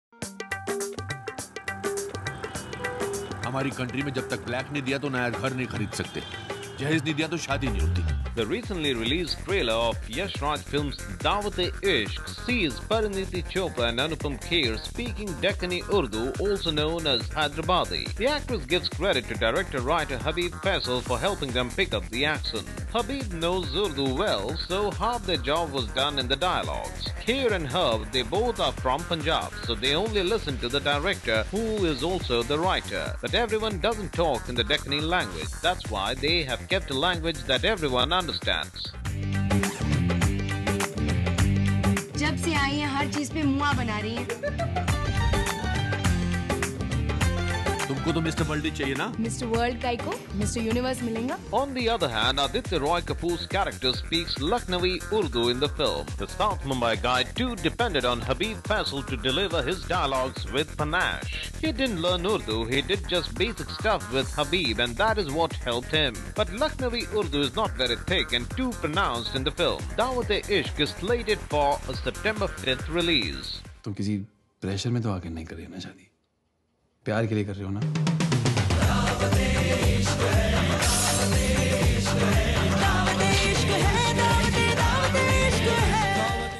Different Types of Urdu | Bollywood Masala | Latest Bollywood News
Watch Latest Bollywood Masala News The recently released trailer of Yash Raj Films' "Daawat-e-Ishq" sees Parineeti Chopra and Anupam Kher speaking Deccani Urdu (Hyderabadi). The actress gives credit to director-writer Habib Faisal for helping them pick up the accent. "Habib sir knows Urdu well so half our job was done in the dialogues. Anupam sir and me, we both are from Punjab, so we only listened to the director, who is also the writer," the 25-year-old said here. But everyone doesn't talk in the Deccani language, that's why we have kept a language that everyone understands," she added. On the other hand, Aditya Roy Kapur's character speaks Lucknowi Urdu in the film.